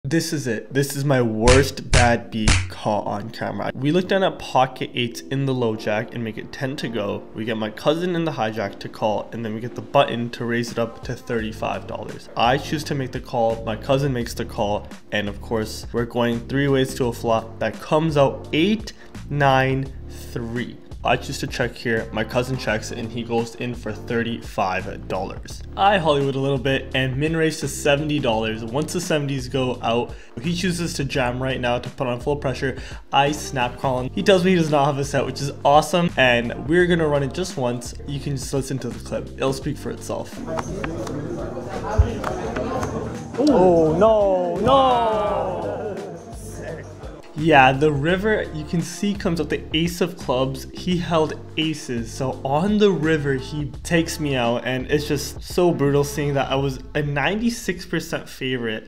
I LOSE as 98% FAVOURITE? HILARIOUS BAD BEAT!
Poker is brutal. I lose as a HUGE favourite to win an ALL-IN pot. Watch me lose chips so you don't have to!

Inspiration: Rampagepoker, Brad Owen, Ashley Sleeth, PokerfaceAsh, Pokerllama, NextGenPoker